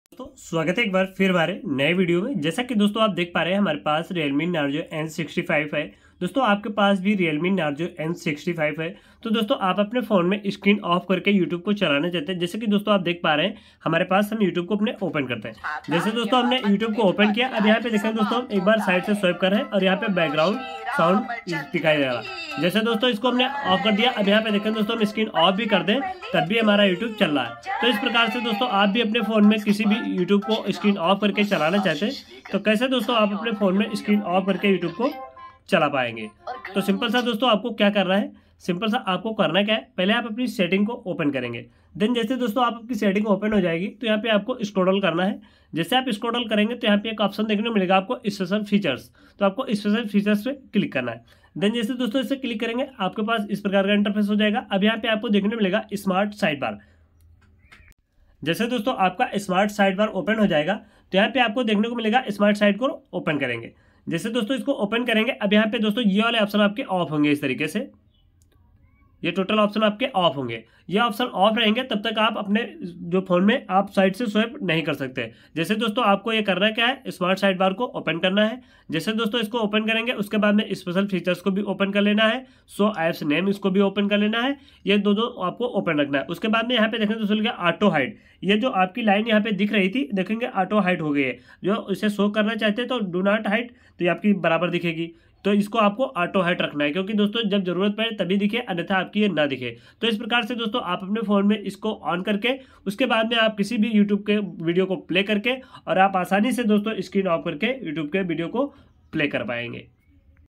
realme narzo n65 screen off karke youtube play kaise rakhe | youtube video mp3 me kaise chalaye
Doston aaj ke is video mein humne aapko bataya ki realme narzo n65 5g mobile me youtube ke video screen lock karke MP3 tarike se kaise chalate hai